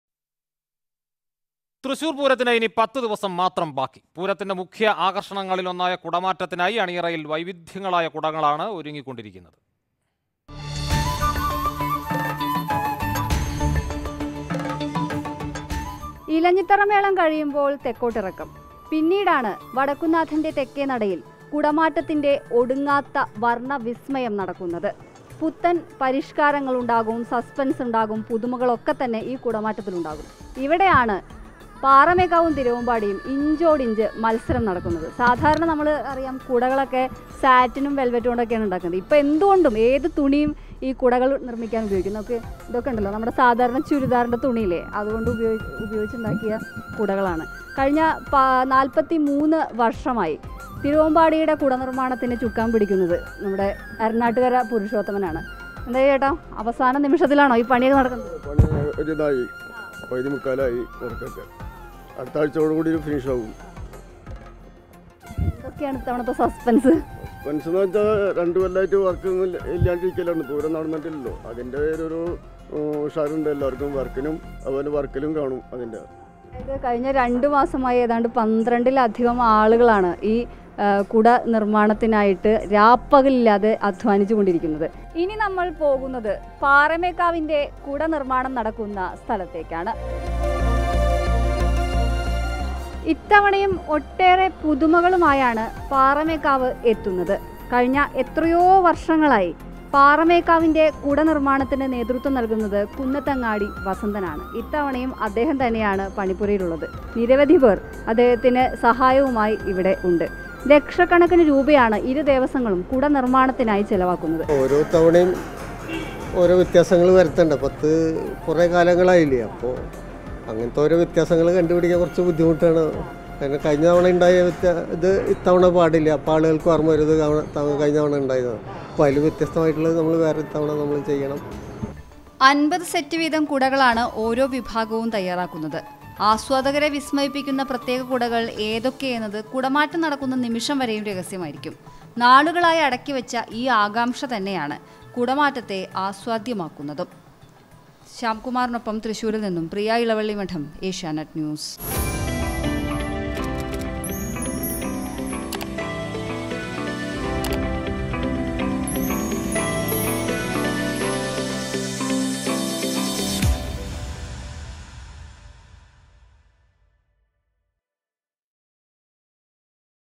പൂരത്തെ വരവേൽക്കാൻ തൃശ്ശൂർ; കുടമാറ്റത്തിനായി അണിയറയിൽ വൈവിധ്യങ്ങളായ കുടകൾ ഒരുങ്ങുന്നു | Thrissur
പൂരത്തെ വരവേൽക്കാൻ തൃശ്ശൂർ; കുടമാറ്റത്തിനായി അണിയറയിൽ വൈവിധ്യങ്ങളായ കുടകൾ ഒരുങ്ങുന്നു.
Thrissur Pooram 2022 |  Umbrellas for Kudamattam